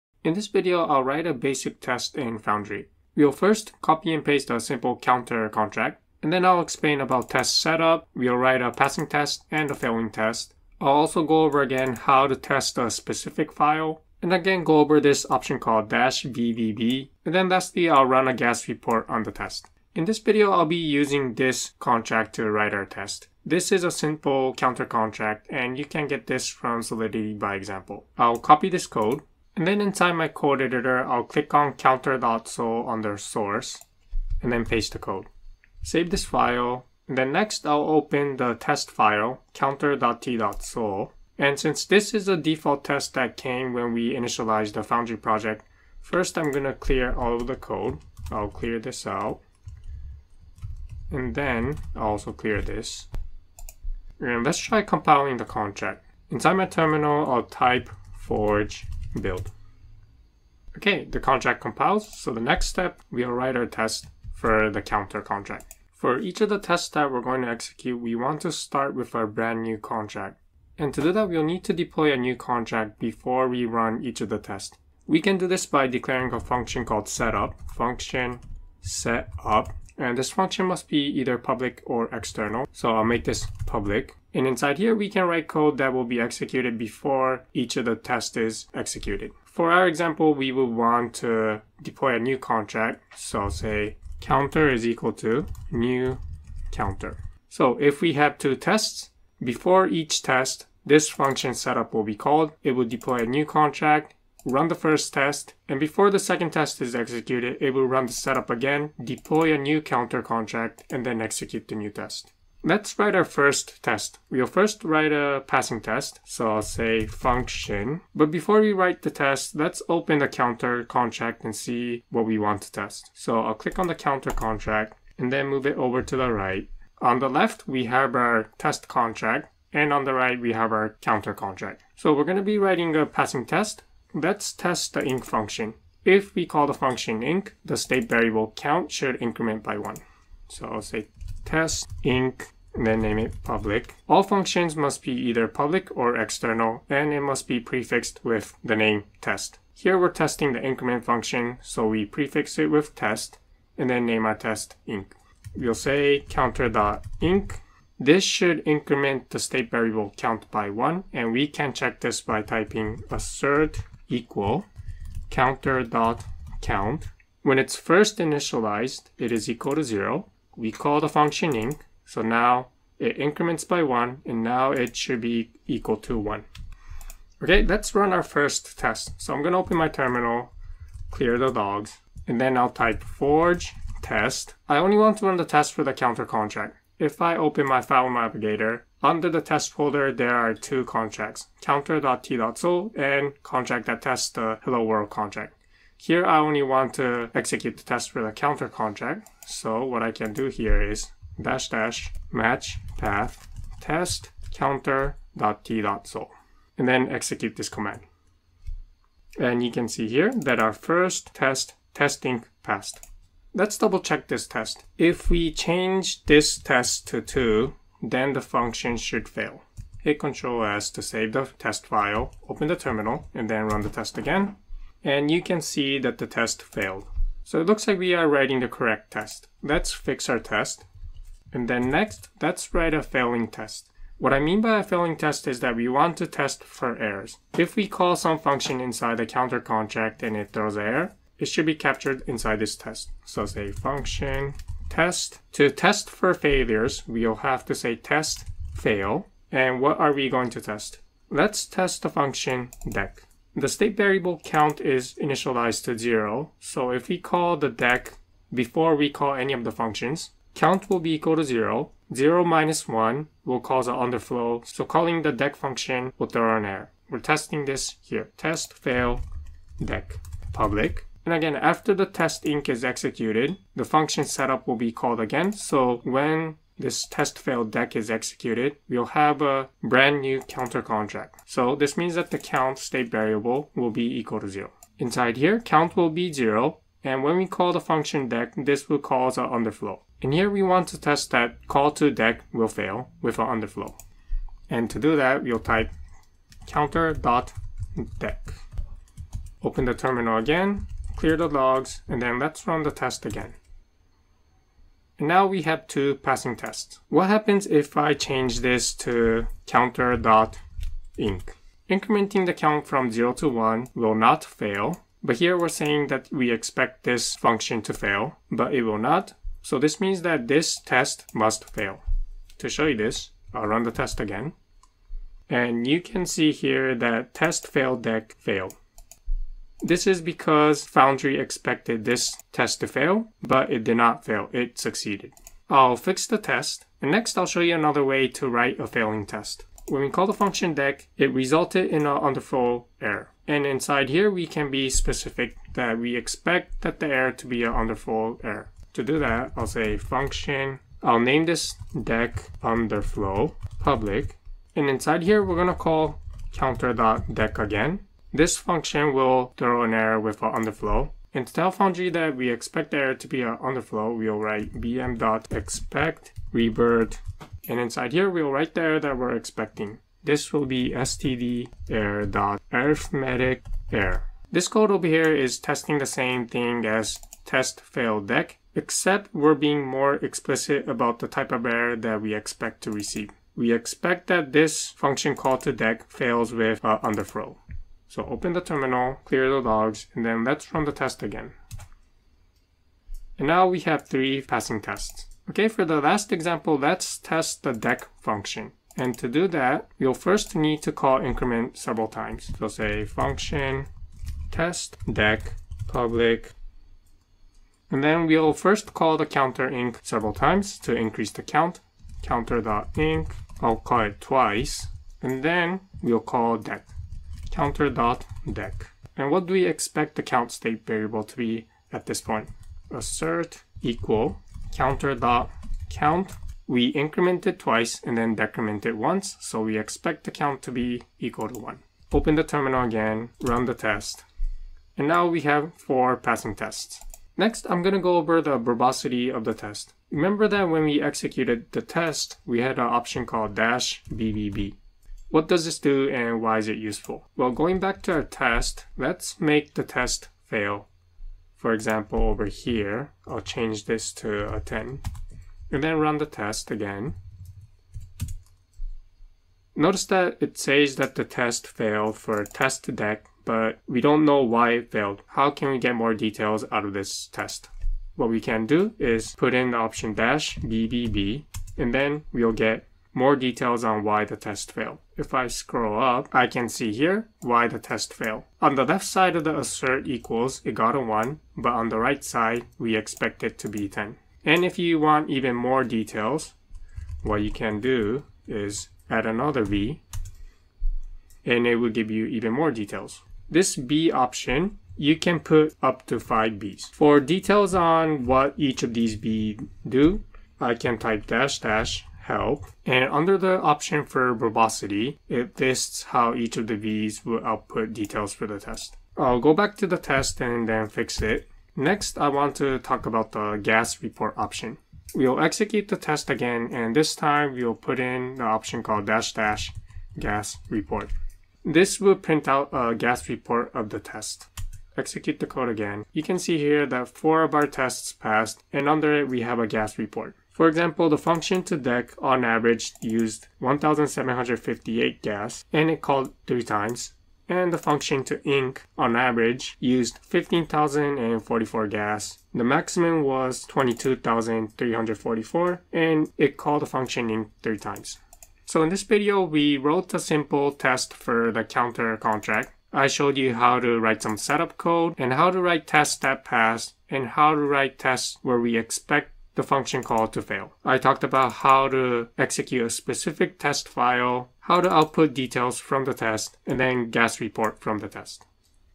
How to Write Basic Tests | Testing with Foundry
How to write basic tests in Foundry

0:00 - Intro
1:00 - Test setup
1:52 - Passing test
3:52 - Test for error
5:37 - Expect revert
6:36 - Test dec
7:17 - Test verbosity
8:31 - Gas report